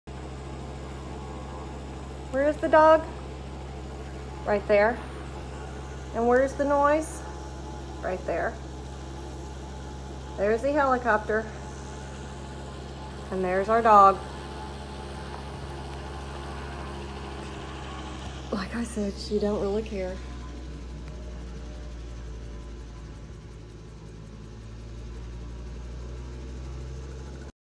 Annie chilling
Our dog is chilling while John Spurling and Okierider in helicopter